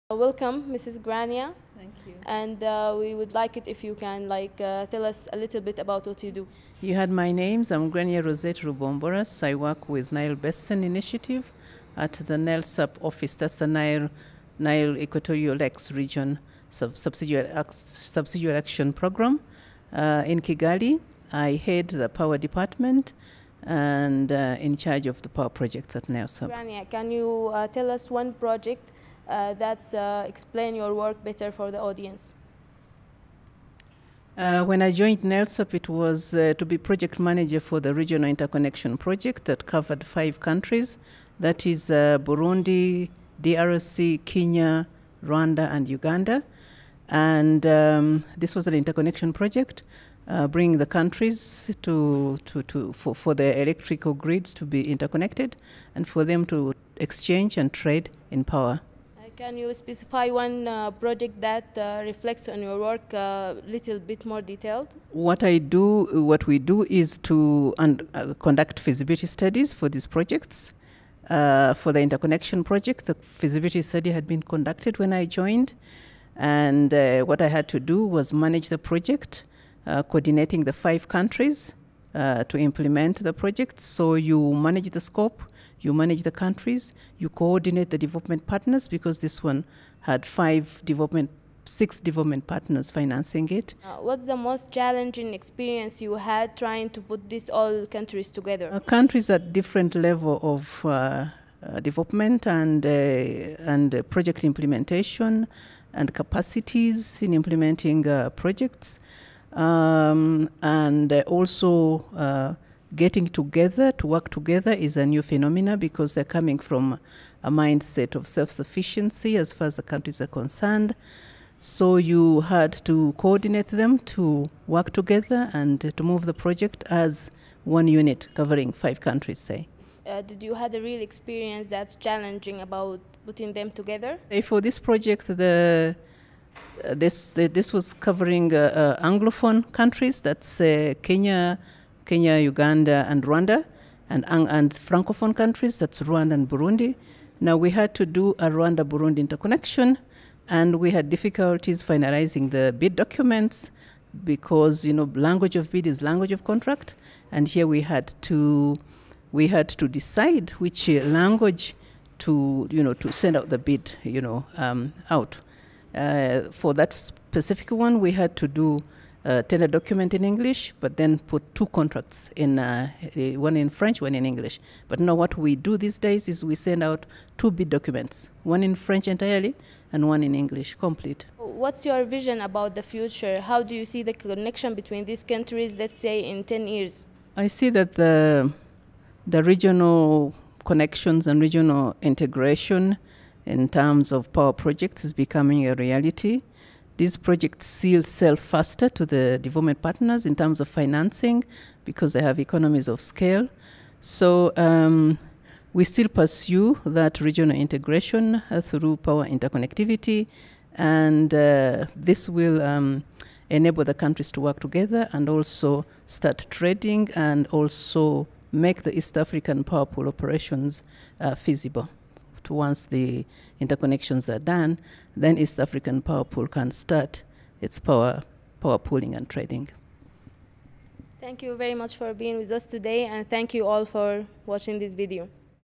The Niles Talk: From one to a million connections, one connection at a time
Elzahraa Jadallah spoke with Grania Rubomboras, Programme Officer at the Nile Equatorial Lakes Subsidiary Action Programme Coordination Unit (NELSAP CU), about her work to light up the region.

(Produced by Media in Cooperation and Transition - MiCT in collaboration with the Nile Basin Initiative and supported by GIZ.)